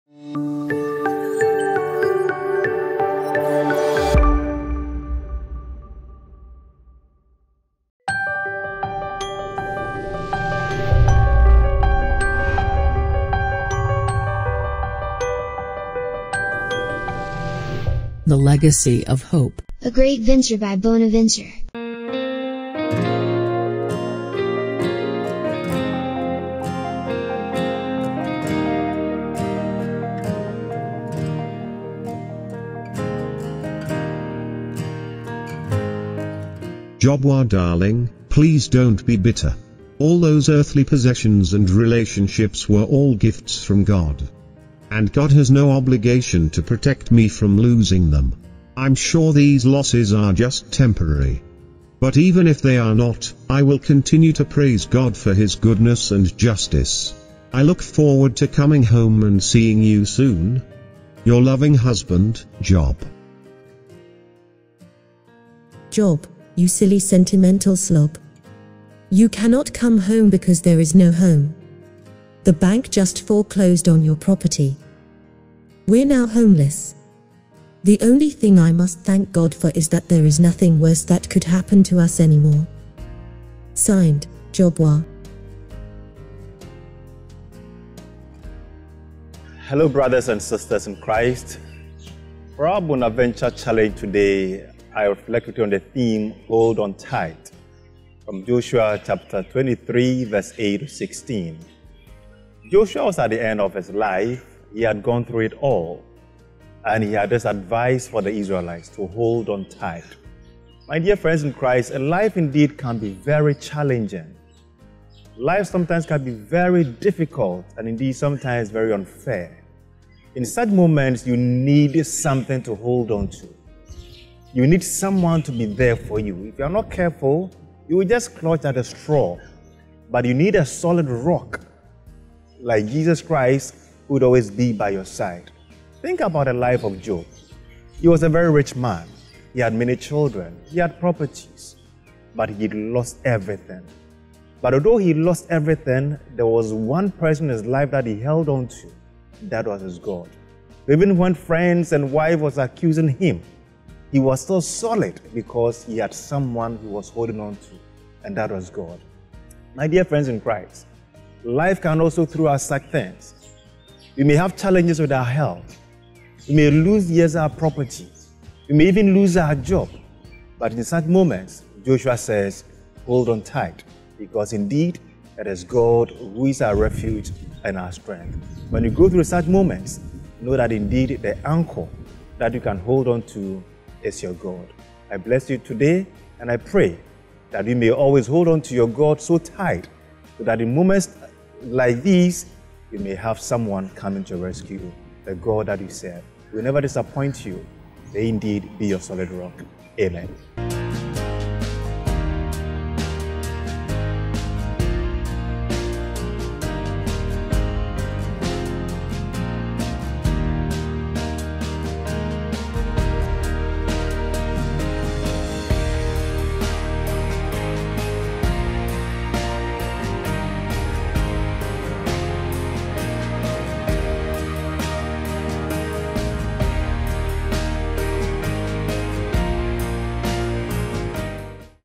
The Bonaventure Lenten Challenge Day5
The theme for Day 5: Hold on tight (Joshua 23:8-16).
The Legacy of Hope project, initiated by Most Rev. John Bonaventure KWOFIE, has become a reality and is, today, a ray of Hope unto posterity.